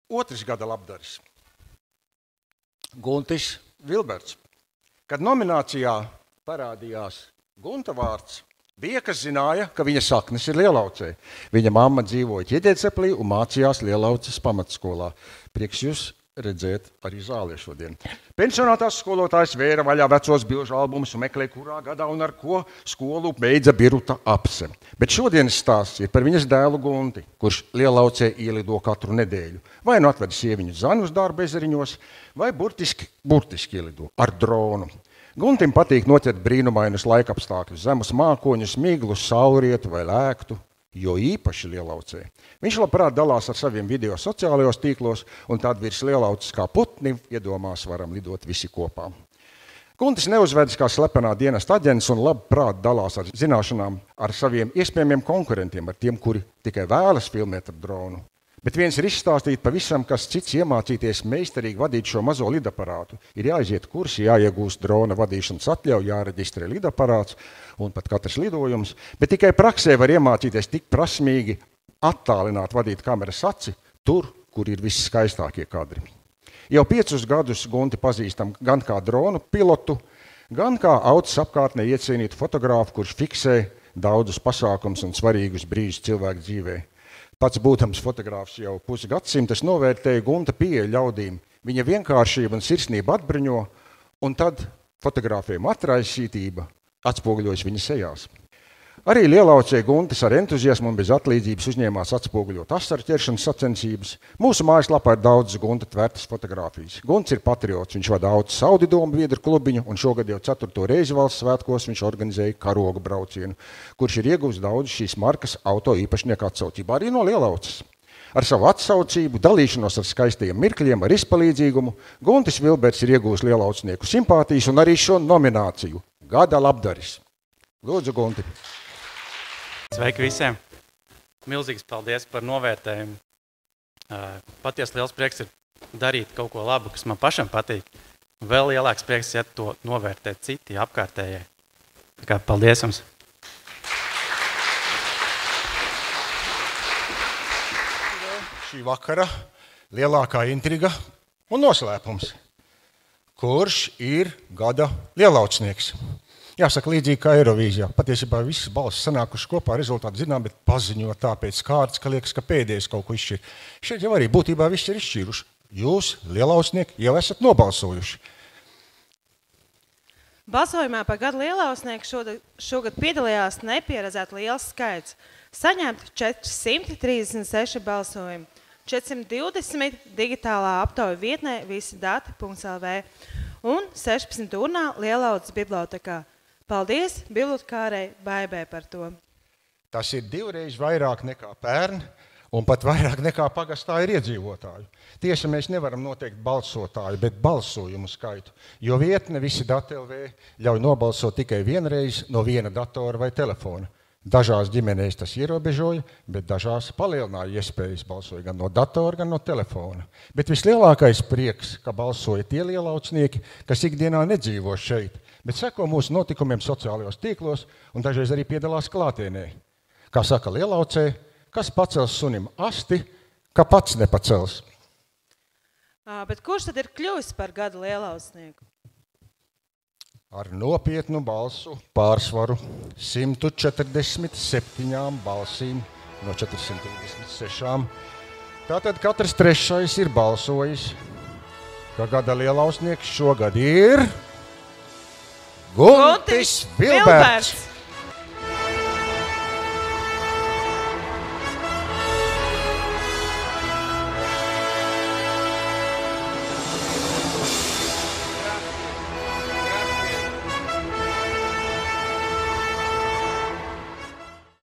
Gada Lielaucnieks 2023 - Guntis Vilberts
"Gada Lielaucnieks 2023" godināšanas ceremonijā Guntis Vilberts tika atzīmēts nominācijā "Gada Labdaris", bet pasākuma noslēgumā noskaidrojās, ka iedzīvotāju balsojuma rezultātā viņš ieguvis arī "Gada Lielaucnieka" titulu un saņēma Dzērves statueti. Video: Dainis Bušmanois